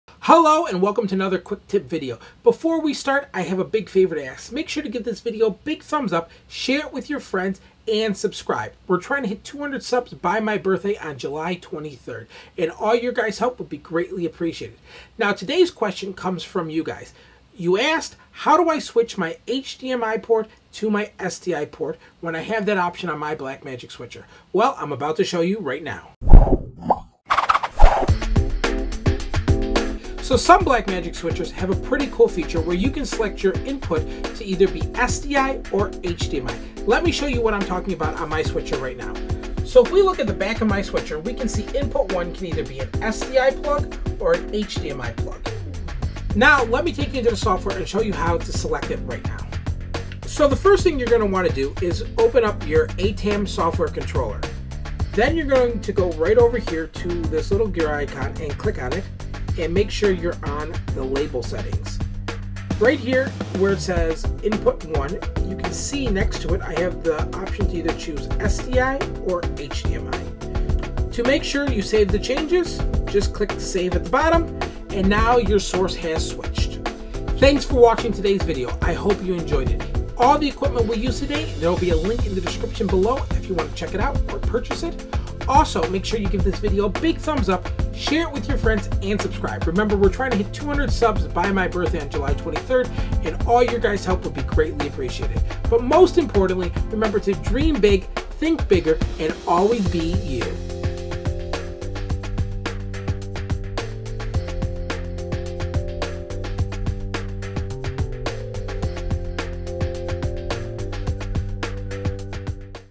How to switch you're BlackMagic Design ATEM Switcher Input settings From HDMI To SDI or SDI to HDMI
This videoWalks you step-by-stepThrough the processOf switching You're Selected input type On your BlackMagic Design ATEM Switcher. NoteIn this example only input one on this switcherIs able to be changed.Not all inputs Are able to switch Between HDMI and SDI
How to switch the input on my black magic switcher
List of equipment in case
ATEM 1 M/E Broadcast Panel
ATEM 2 M/E Production Studio 4K
SmartView HD
HyperDeck Studio

Send Mail To:
257 Northfield Road